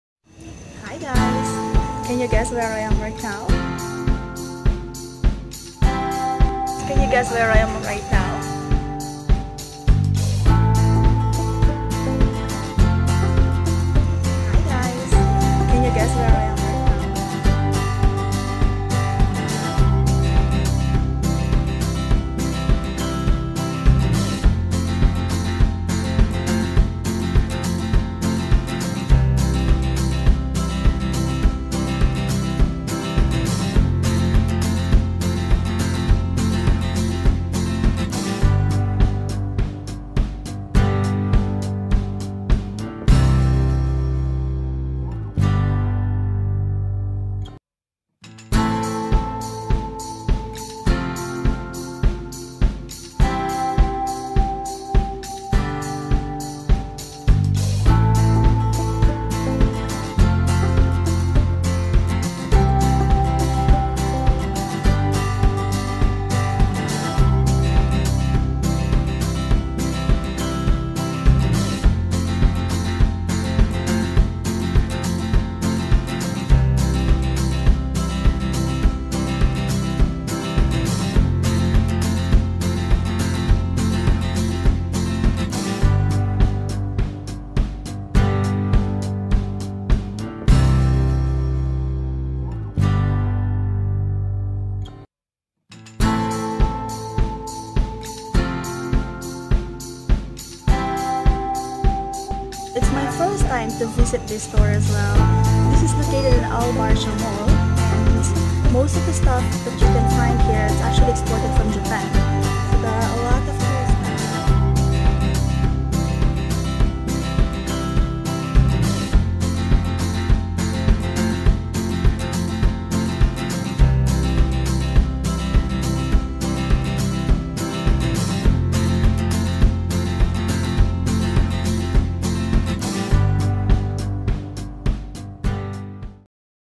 Visiting Daiso Japan in Al Barsha Mall
The much-loved retail concept began as a Japanese street vending operation focusing on 100-yen products by Mr Hirotake Yano in 1972. The simple, yet effective, philosophy of Daiso came when he was working as a salesman, "There were so many people buying, demanding to know the price immediately, that I found it difficult to keep up" he reminisces. “That’s when I thought of a store where all items will be sold at the same price.” Sounded simple enough and Mr Yano decided to turn that idea into reality. As the business flourished, the brand Daiso was founded in 1977, and has not looked back since.